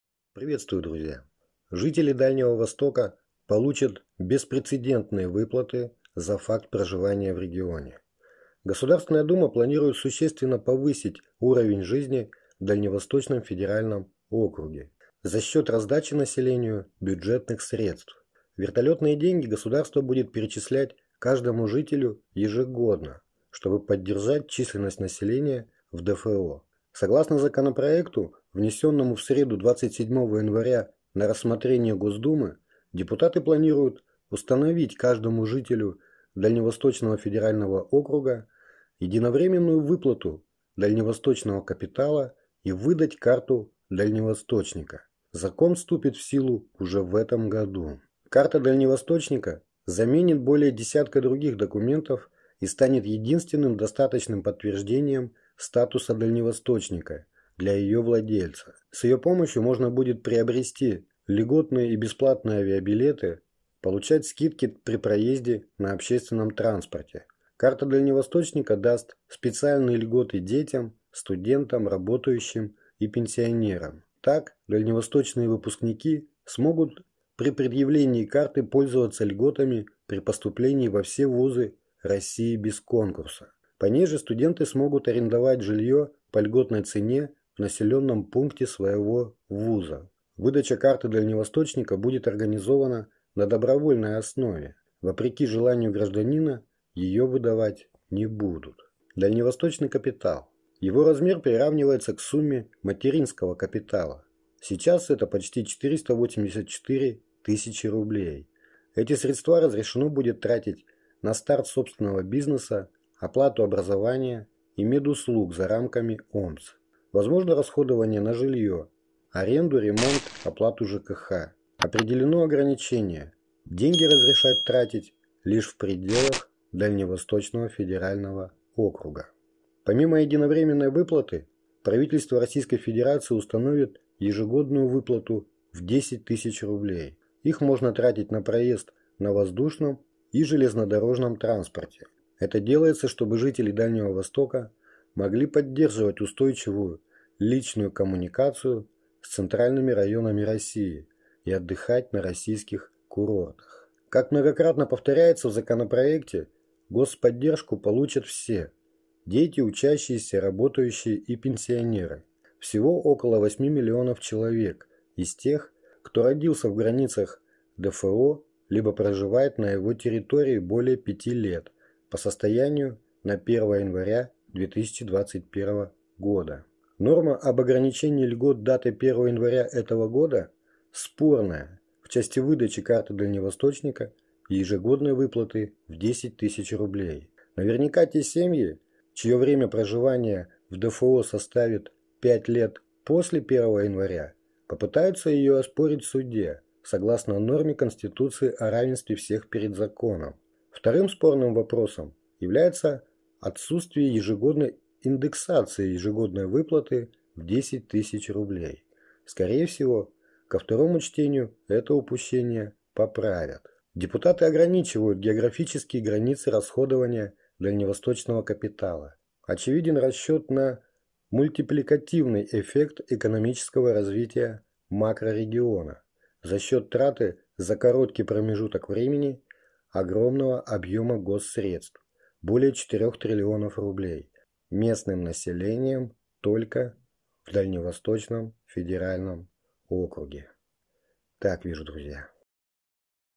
Жители Дальнего Востока получат выплаты карта жителя ДФО дальневосточный капитал новости сегодня
В государственную думу российской федерации внесен законопроект о поддержке жителей Дальнего Востока. Он предусматривает ежегодные выплаты всем без исключения жителям ДФО выплаты в размере 10000 рублей. Также в законе прописано введение так называемой льготной карты дальневосточника, а также дальневосточного капитала. Примечательно, что эти деньги можно будет потратить только на территории ДФО, что безусловно насытит край деньгами.
                Очень хорошо, что появились подвижки в борьбе с убылью населения Дальнего Востока.

                Источник riafan.ru